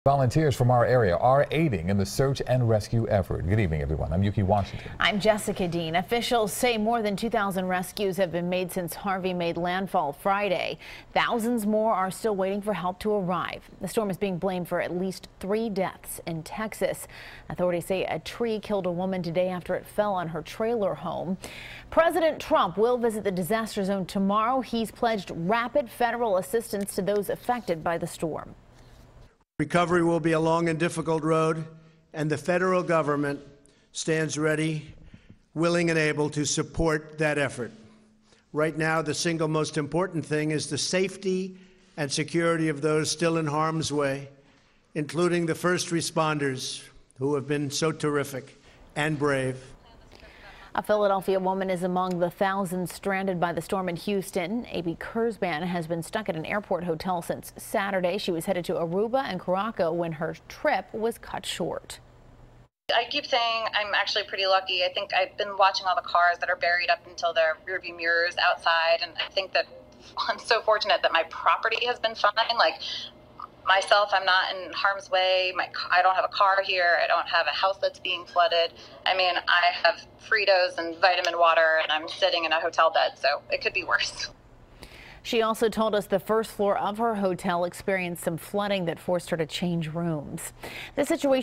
Trump Pledges Rapid Federal Aid To Victims Of Hurricane Harvey
President Trump will visit the disaster zone on Tuesday.